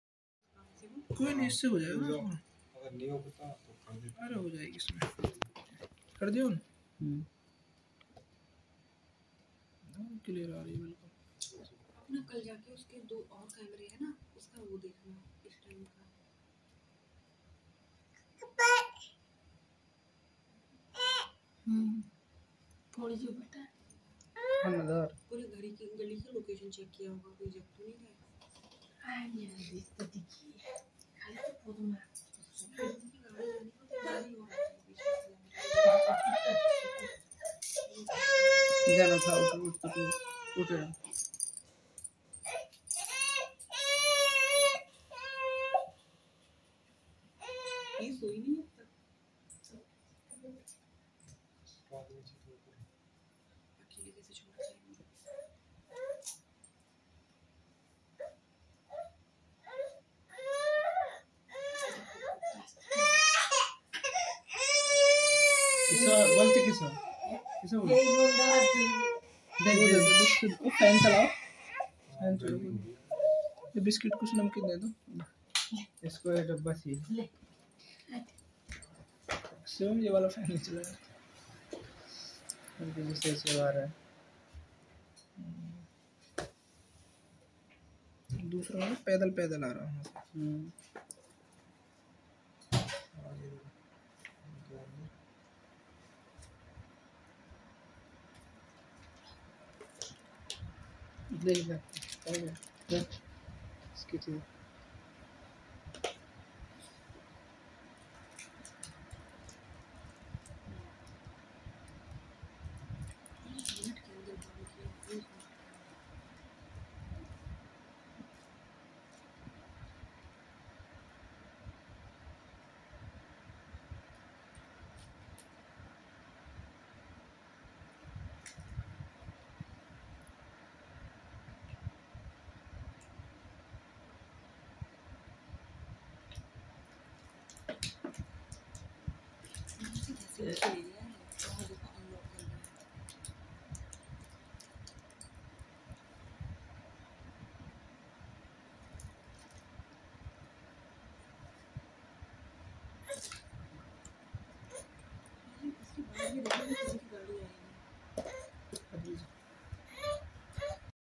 bike chore kortey huey cctv mai record
Greater Noida sector-36 F-18 k samne se splender UP16 CM 3504 Bike ko le jate huey CCTV  mai record.. 4 sep 2021